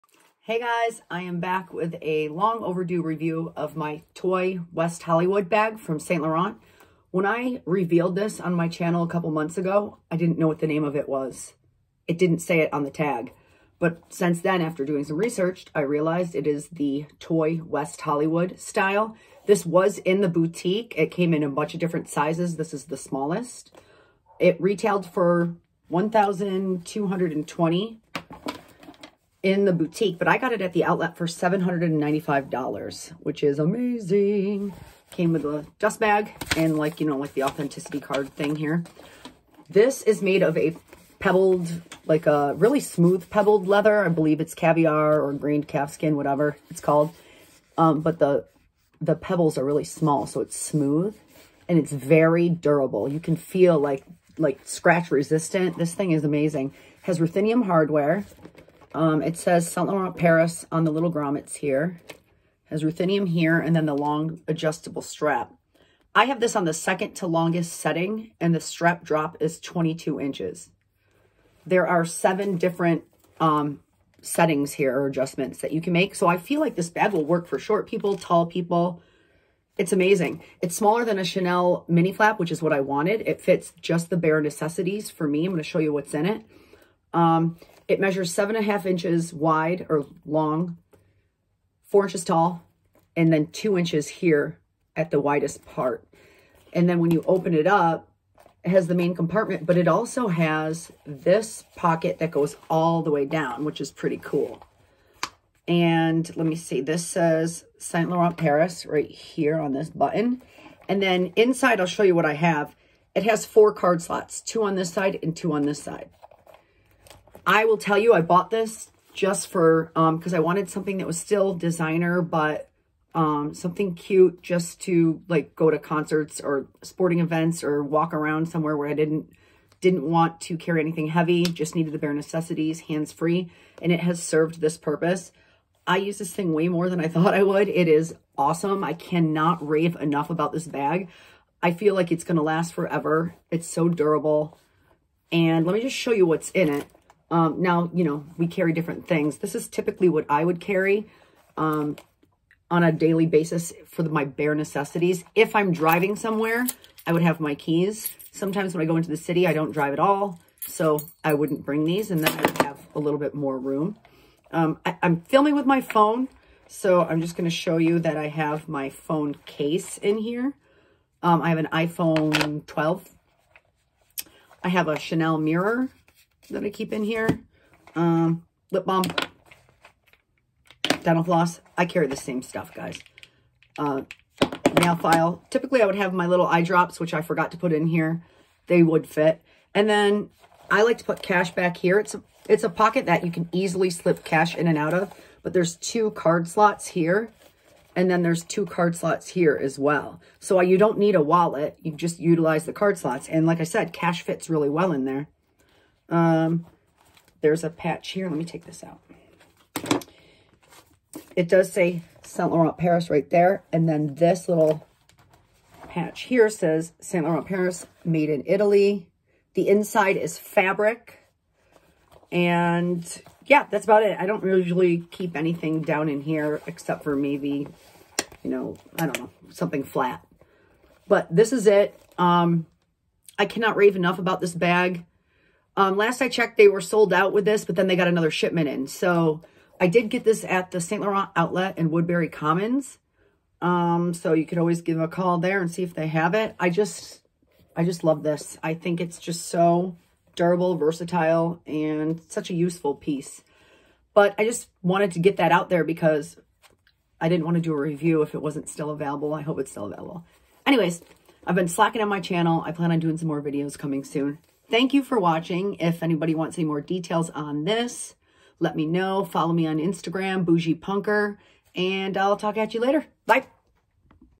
Review and what fits in the Saint Laurent Toy West Hollywood bag
Purchased from the YSL outlet for $795 plus tax.